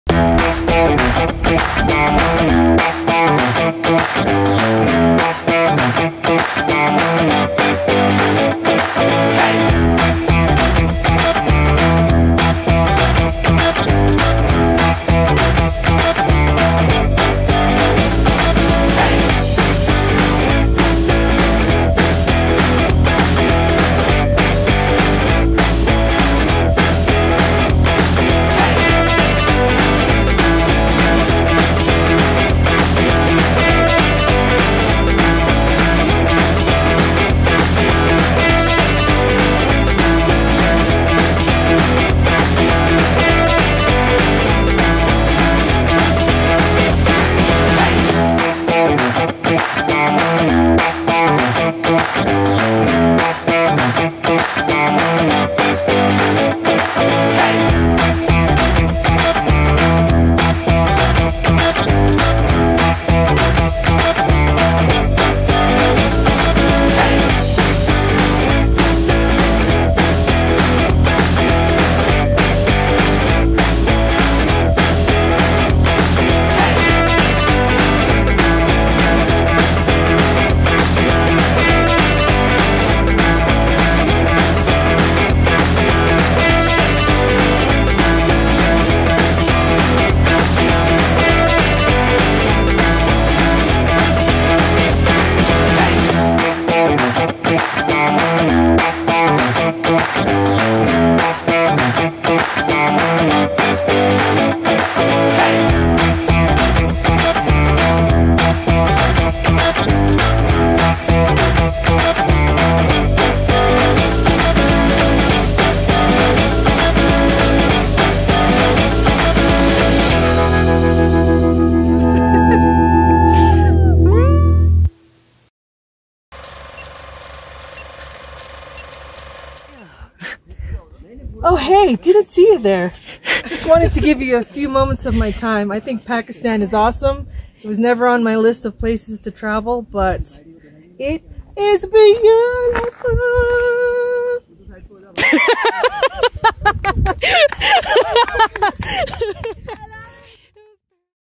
Pakistan women's tour | Female travelers visit Pakistan for the first time
Who says women can't travel to Pakistan? Here's some highlights from our 2019 women-only tour of Pakistan, when I collaborated with The Mad Hatters to take female travelers from 5 different countries to experience a feminine side of Pakistan few first-time visitors get to see.

The fun doesn't stop there: our tour went beyond the standard sights to offbeat areas few tourists visit, staying in local homestays and using local female guides whenever possible. Responsibility matters!